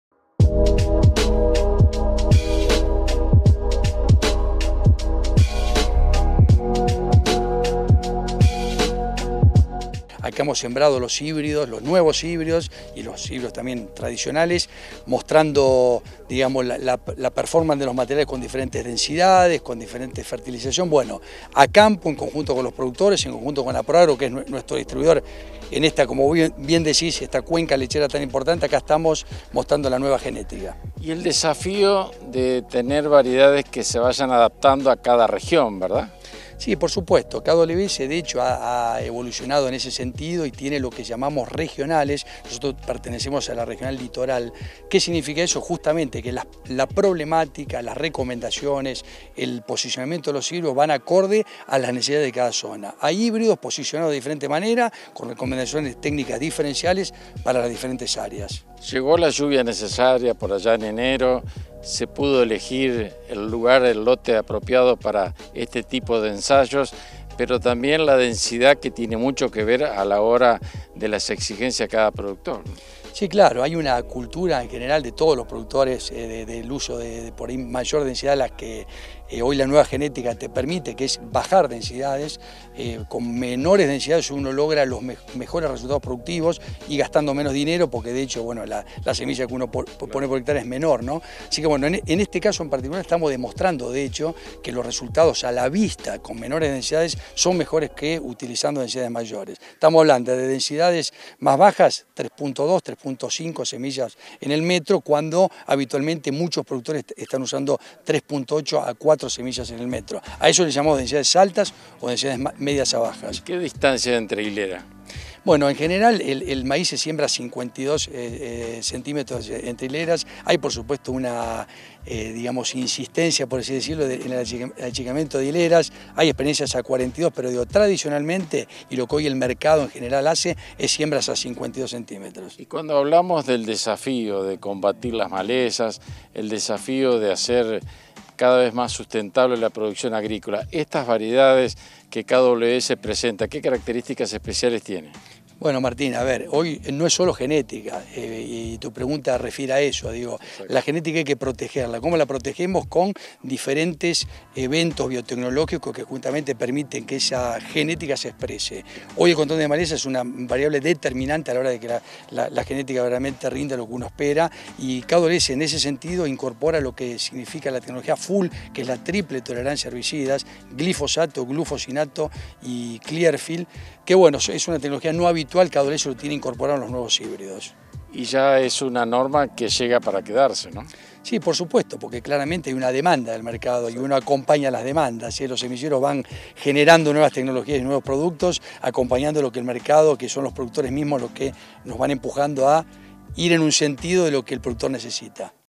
MBTV || KWS Argentina presentó su porfolio en una jornada técnica a campo
Martín Bustamante TV dialogó con Marcelo Chiaraviglio, responsable técnico comercial de KWS para el área Centro Norte de la provincia de Santa Fe. Junto a los productores, la firma líder en genética de vanguardia, presentó su nuevo porfolio de productos en un lote demostrativo en la zona rural de Galisteo, Humberto Primo.